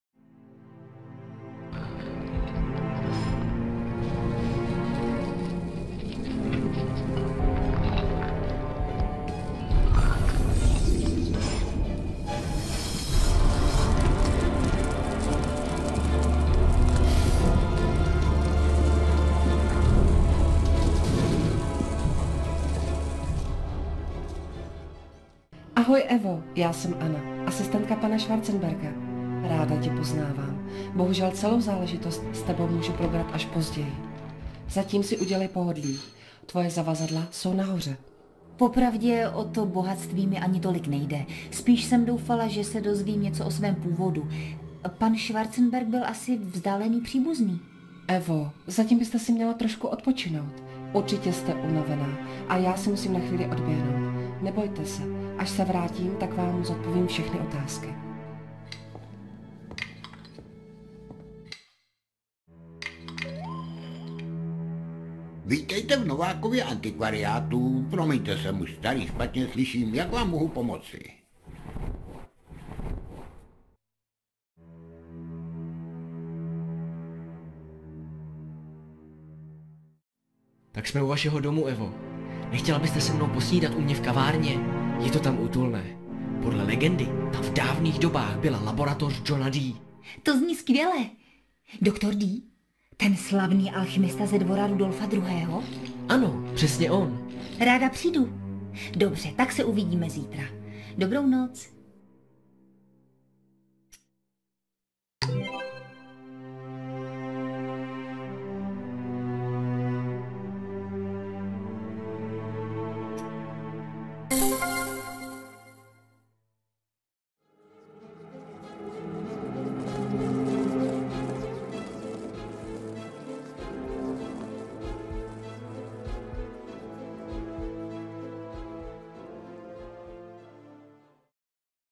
Praha magická
Jaká skvělá zpráva! Do klína vám spadla luxusní vila v samotném centru Prahy. Brzy však zjistíte, že honosné sídlo po staletí ukrývá temné tajemství. Dokážete uniknout vábení zla